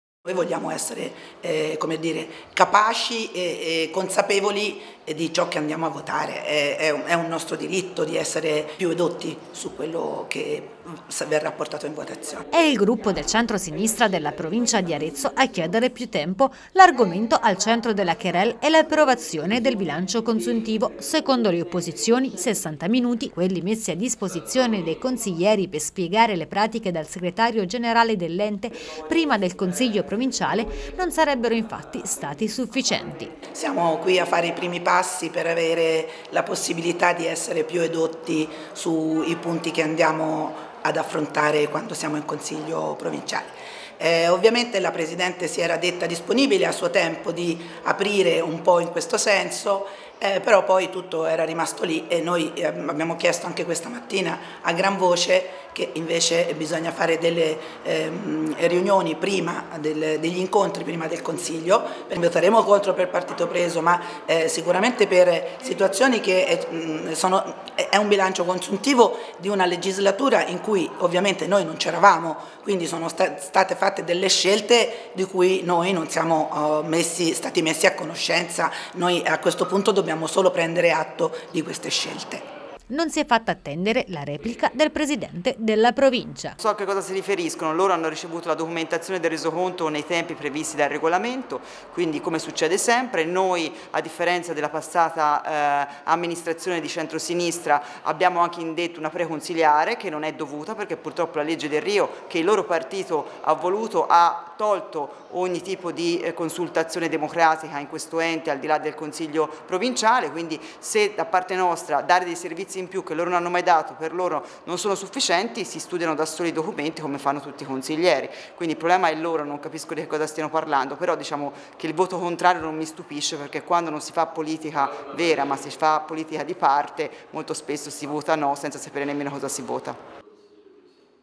La replica del presidente della Provincia sulla richiesta del gruppo del centro sinistra
È  il gruppo del centro sinistra della Provincia di Arezzo a chiedere più tempo. L'argomento al centro della querelle è l'approvazione del bilancio consuntivo.

Secondo le opposizioni 60 minuti, quelli messi a disposizione dei consiglieri per spiegare le pratiche dal segretario generale dell’Ente prima del consiglio provinciale, non sarebbero stati sufficienti.
Non si è fatta attendere la replica del presidente della Provincia.